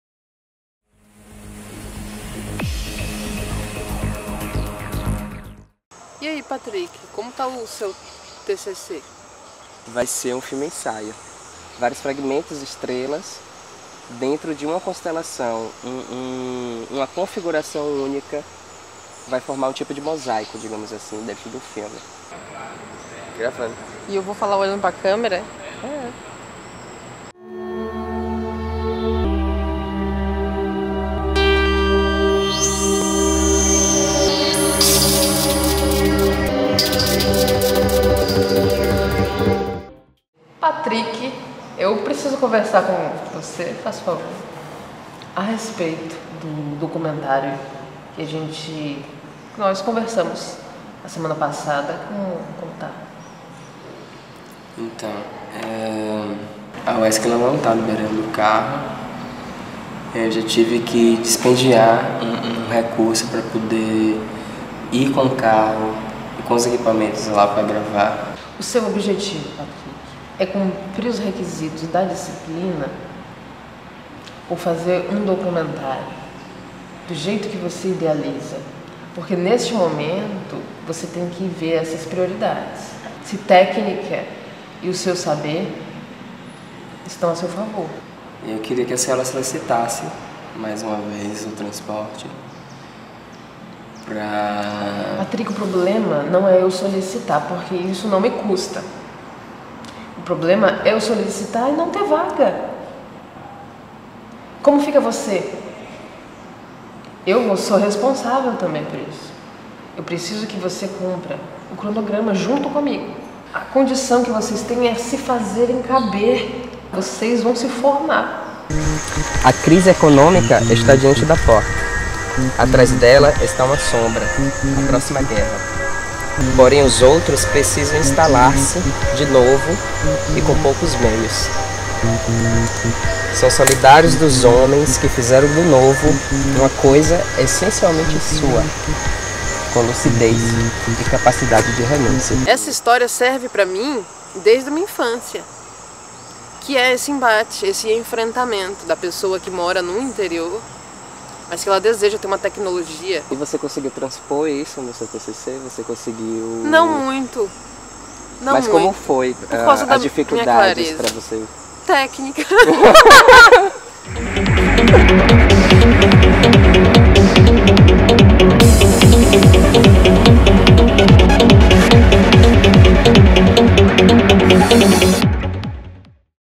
Ensaiando a Forma do Roteiro Filmado
Trata-se de um produto em um sub-gênero quase inexistente na cinematografia brasileira que é o "roteiro filmado", cujo maior expoente foi o cineasta francês Jean-Luc Godard. No vídeo, experimento fazer um roteiro filmado do meu produto de TCC intitulado "Passagem", o qual tratará da exposição ficcional de minhas experiências quando da minha passagem pela universidade.

Participante: PATRICK SILVA CAVALCANTE